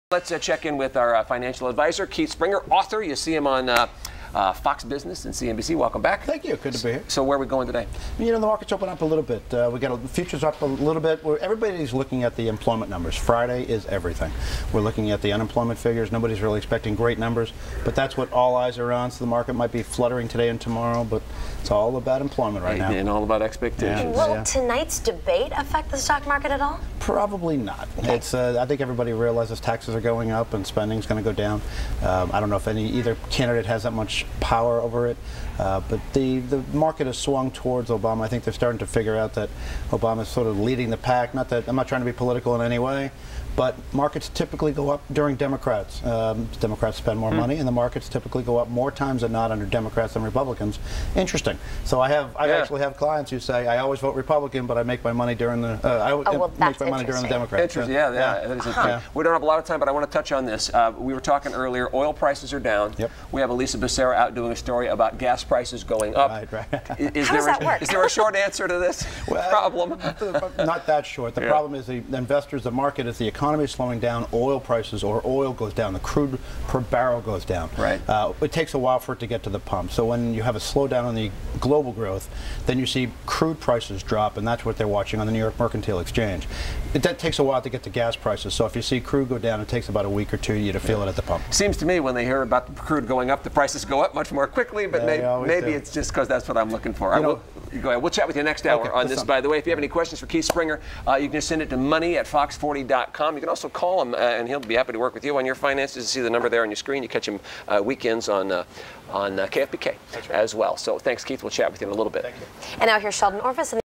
Keith Springer on Fox 40 10-3-12 5:45 AM
Keith Springer, President of Springer Financial Advisors is the NEW FOX 40 FINANCIAL ANALYST at 5:45 am, 6:45 am and 7:20 am, every day! Watch Kieth's 5:45 am clip HERE!